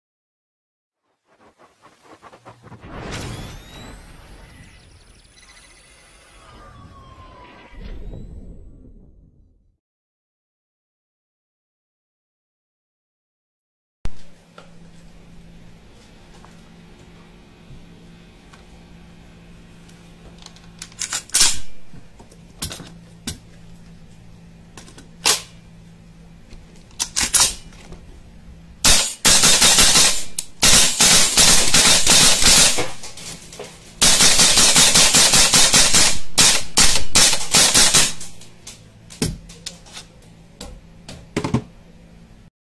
VFC MK12 MOD1 GBB DX 2015 Quick Demo by WGC Shop
Another quick demo by WGC Shop, this time its the 2015 version of the VFC MK12 MOD1 GBB DX (SPR). This has fully licensed trades from Colt, and as always the external details of the rifle is really good, just how the way VFC does it.